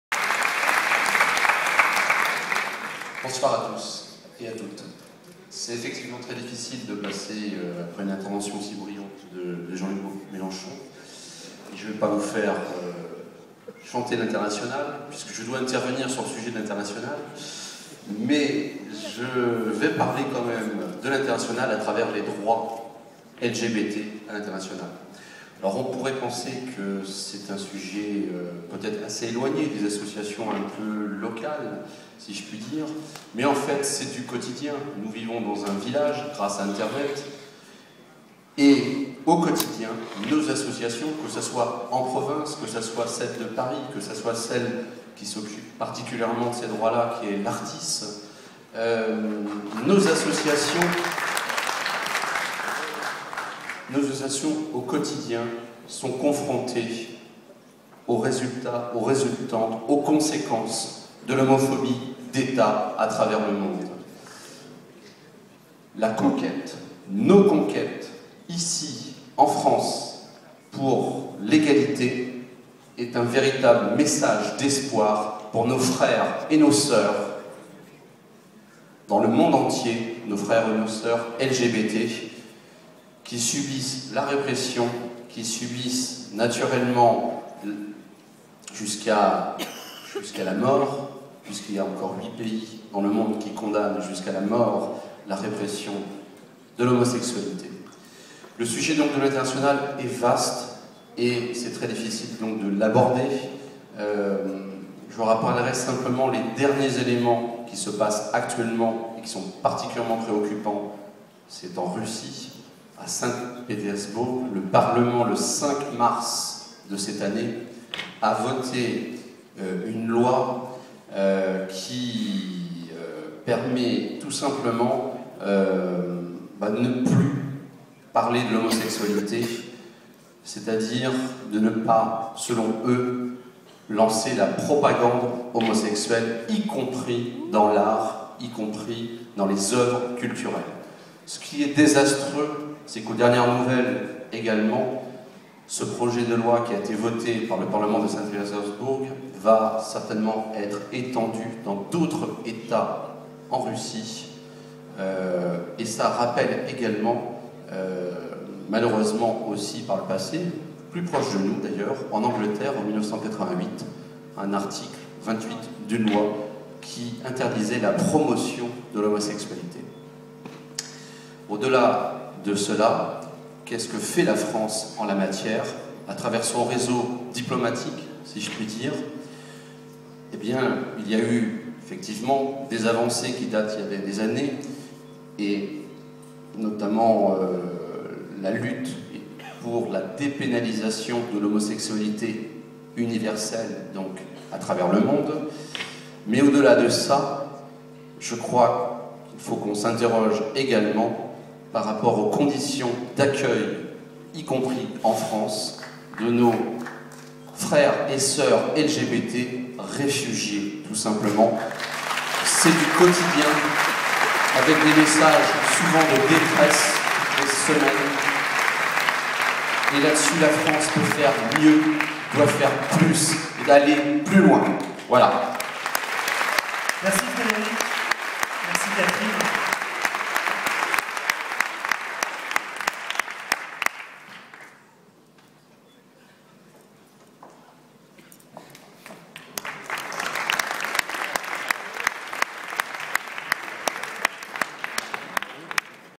Questions internationales sur les sujets LGBT
Frédéric Hay présente les questions internationales LGBT lors du Meeting pour l'Egalité.
Vidéo à retrouver également sur l'application Gay Live.